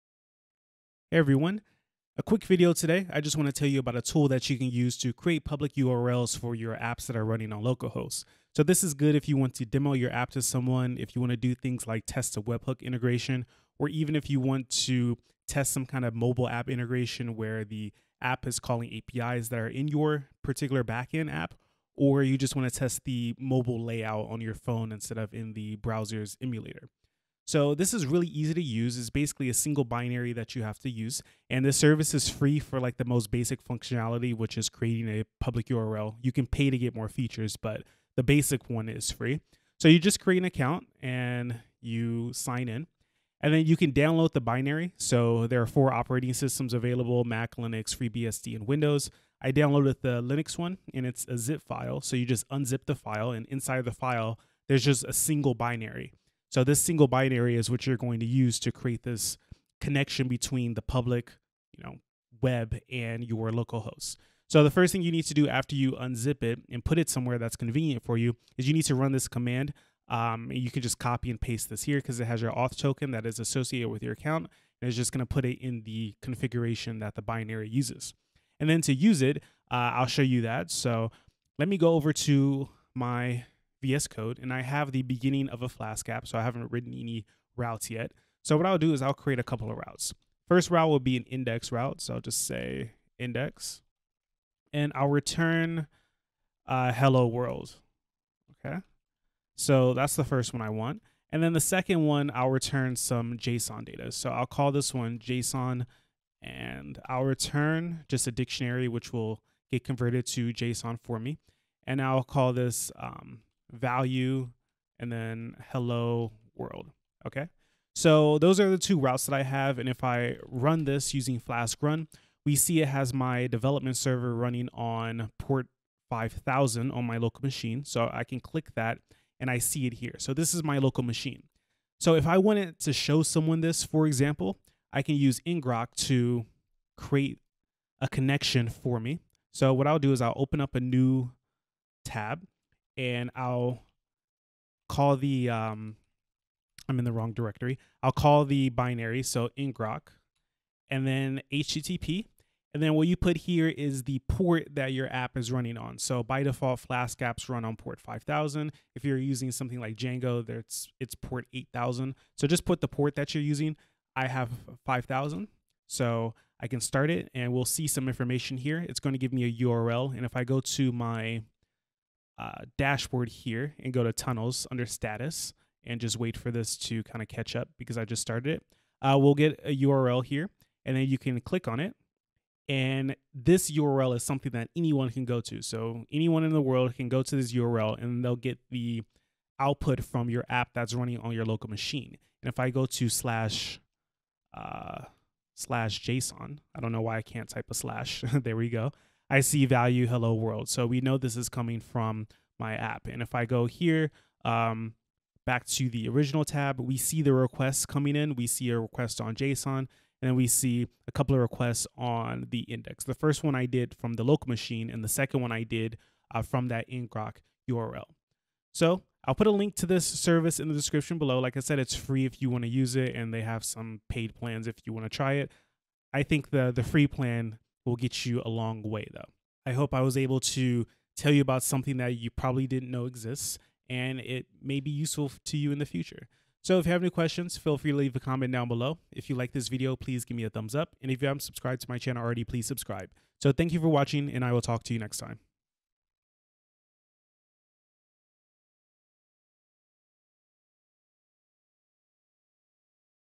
A Method for Creating Public URLs for Your Localhost Web Apps
Use ngrok.io to create public urls for web apps running on localhost.